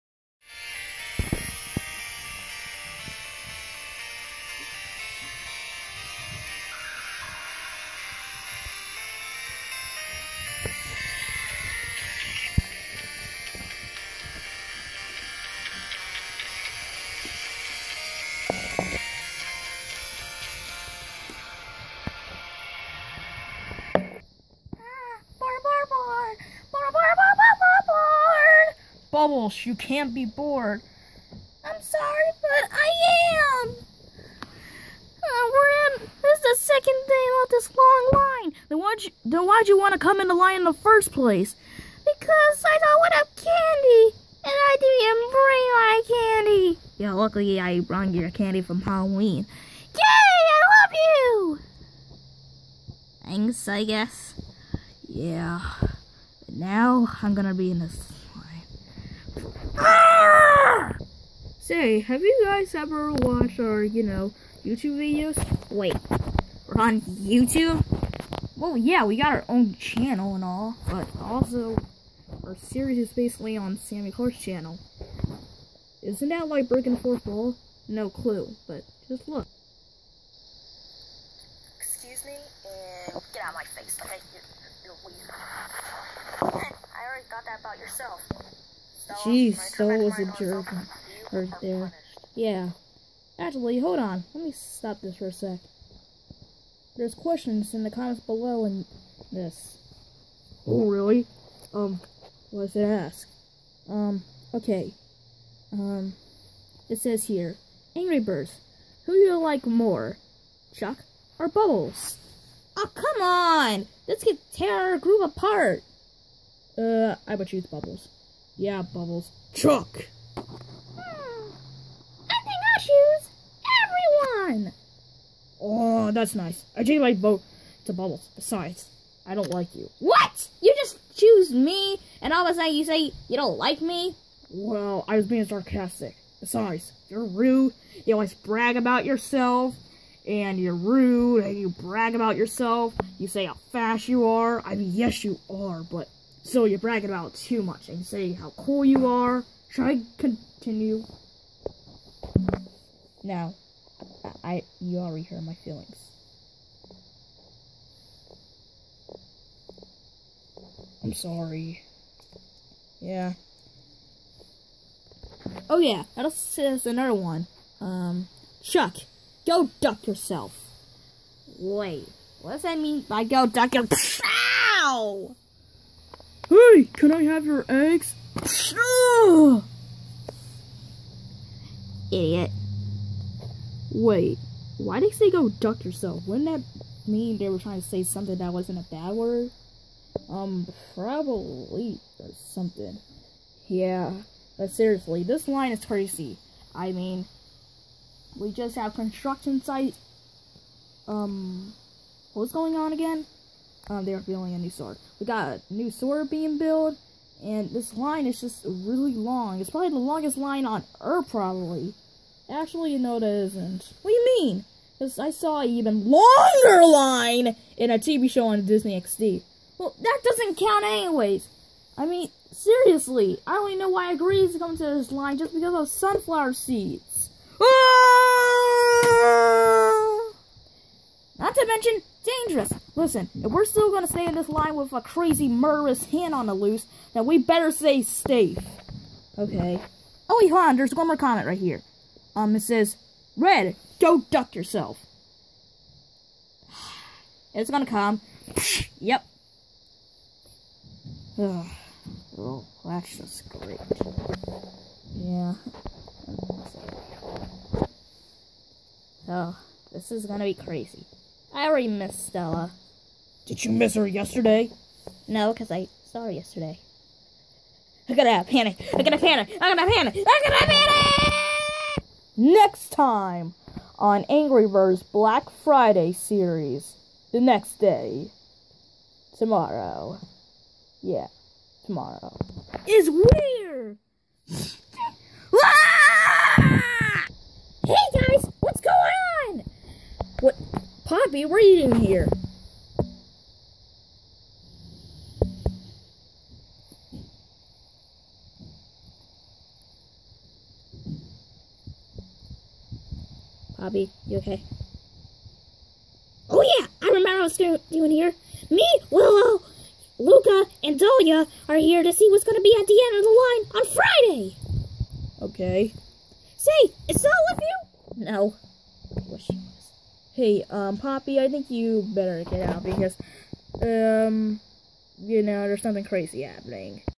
Angry Birds Series Episode 11: BLACK FRIDAY DAY 2
The Birds are still waiting in line and they answer some questions! P.S. These questions that are asked aren't real cause I was coping off Annoying Orange BLACK FRIDAY series from last year.